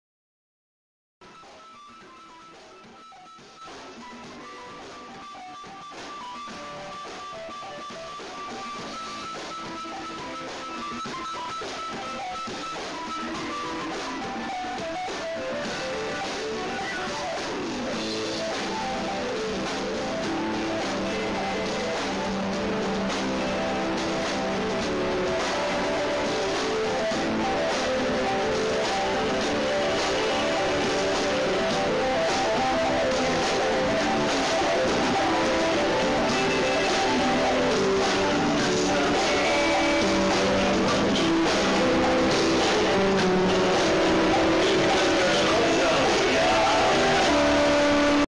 Firci.mpeg
Firci,Kornelije Kovac,Cima,Marko,Tmomke,Jani proba pred koncert